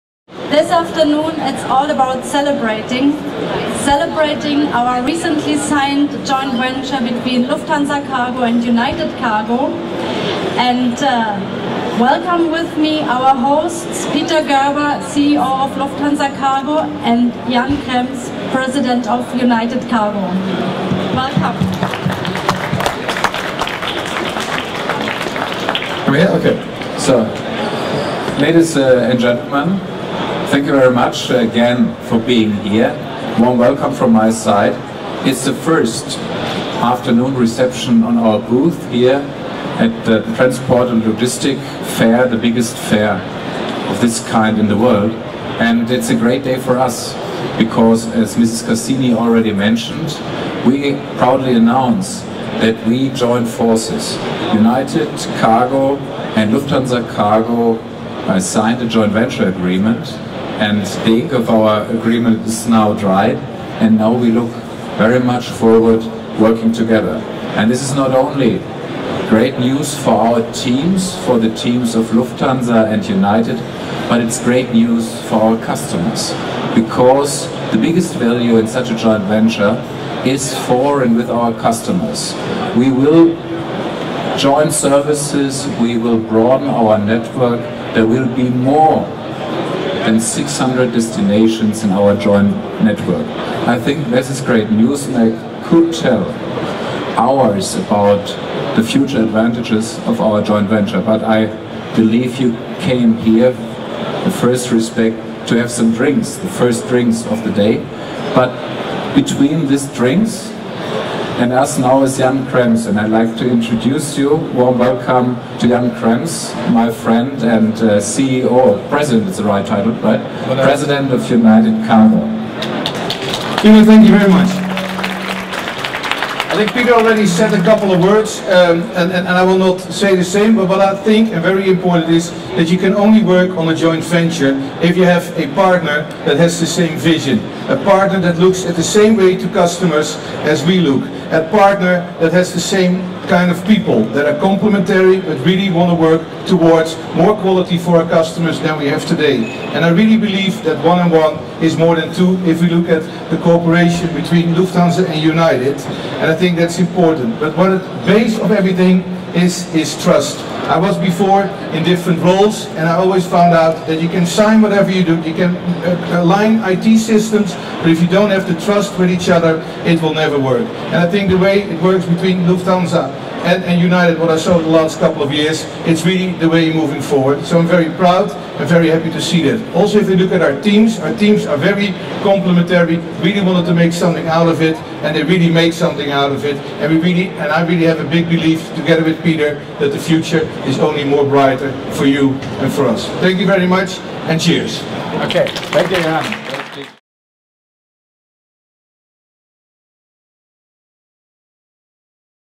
Lufthansa Cargo and United Cargo Celebrate Joint Venture at Transport Logistic in Munich
Watch Jan Krems, President of United Cargo, and Peter Gerber, CEO of Lufthansa Cargo, ring in the celebration of their recently established cooperation at transport logistic in Munich.